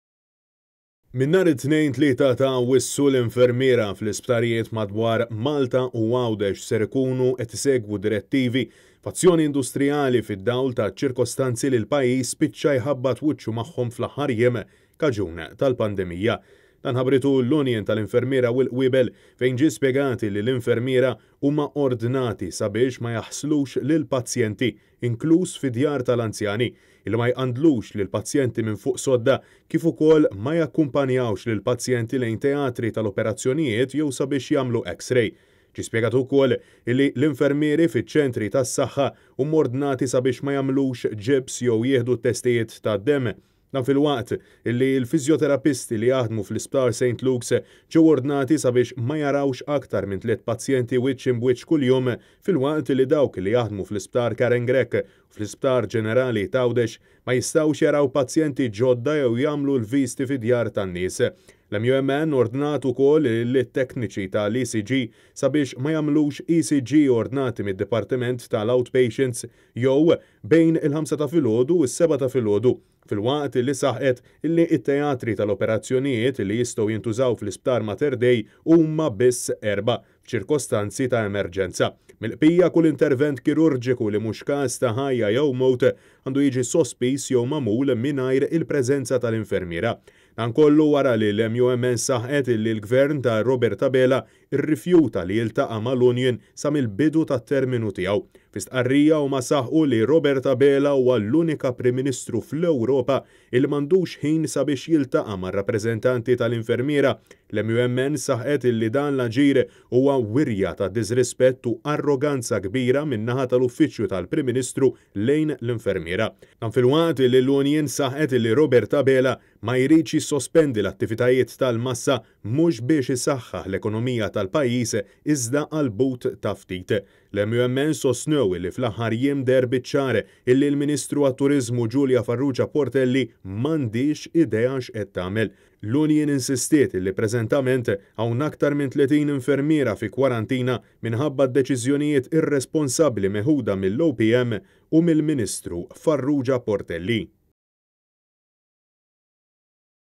L-infermiera b'azzjonijet industrijali
Minn nhar it-Tnejn, 3 ta' Awwissu - l-infermiera fl-isptarijiet madwar Malta u Għawdex ser ikunu qed isegwu direttivi f'azzjoni industrijali fid-dawl taċ-ċirkostanzi li l-pajjiż spiċċa jħabbat wiċċu magħhom fl-aħħar jiem.